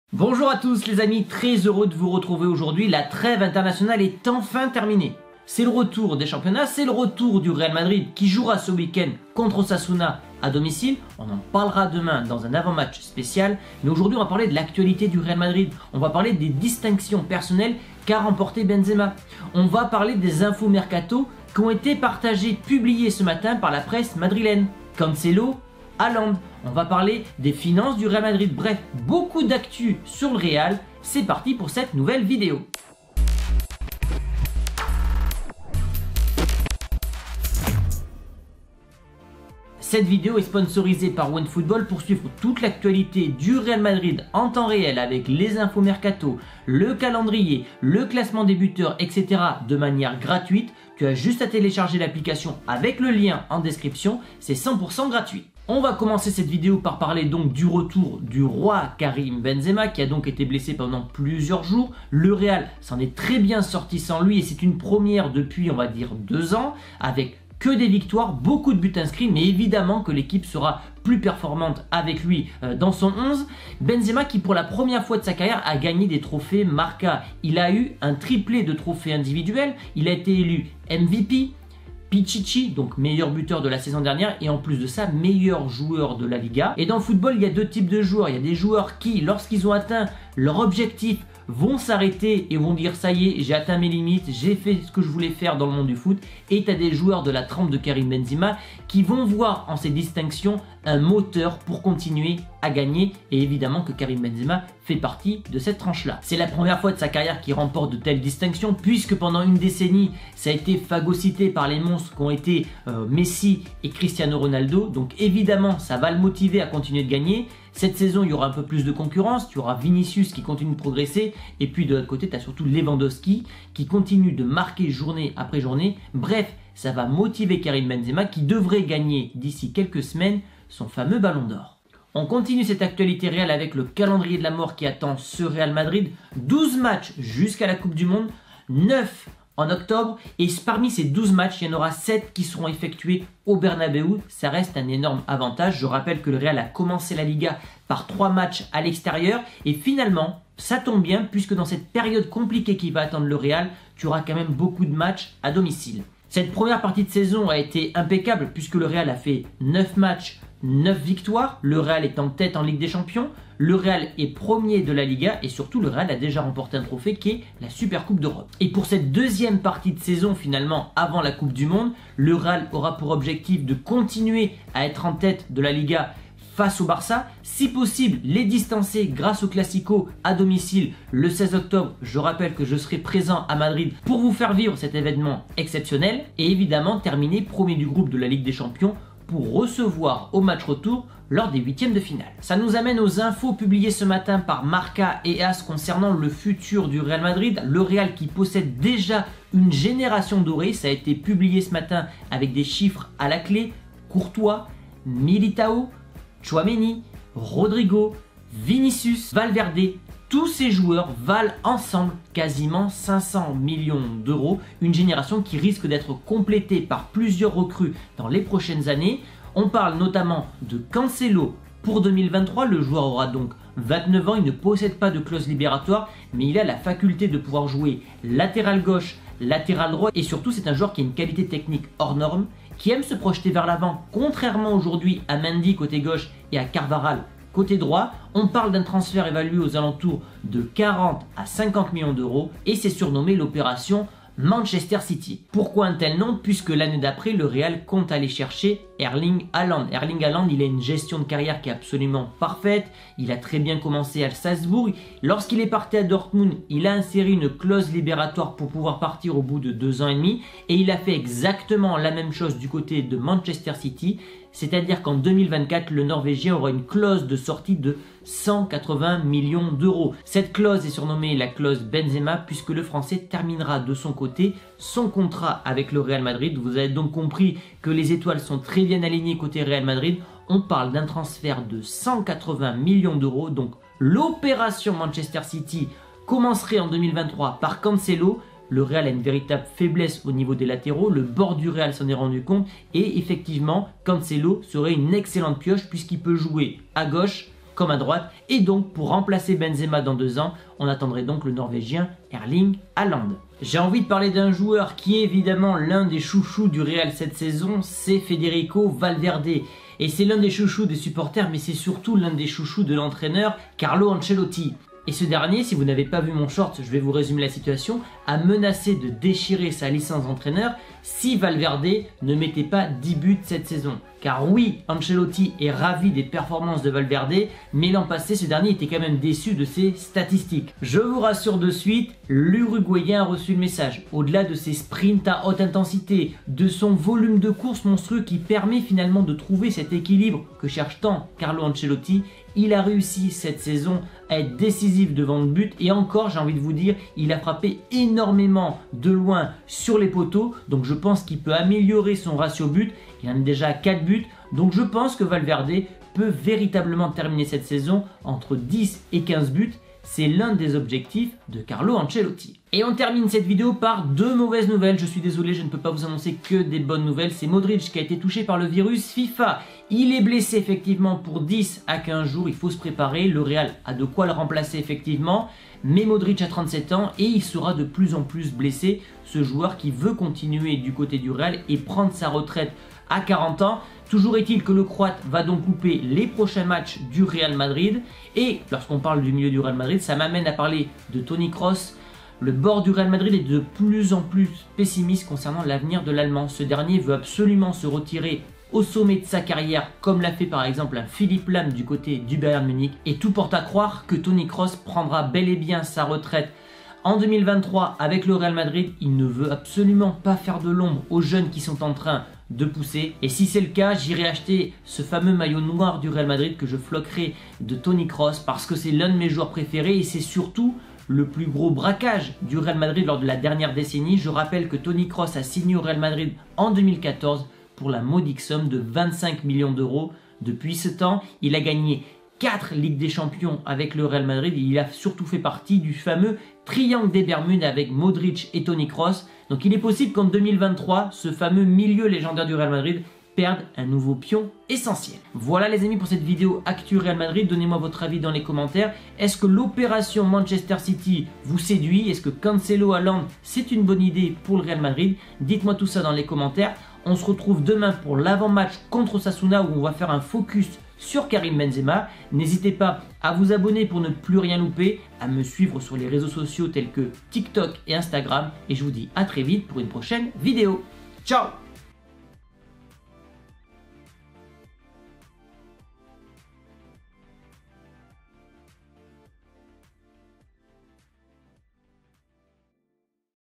🔥Le Real lance l'opération ''City" : Cancelo en 2023 et Haaland en 2024
00:00 Introduction
00:55 Benzema reçoit 3 titres MARCA
02:12 Le calendrier atroce qui attend le Real Madrid
03:12 Cancelo 2023 et Haaland 2024 : opération City
05:03 Valverde le pion essentiel d'Ancelotti
06:02 Modric , Kroos vont mal ?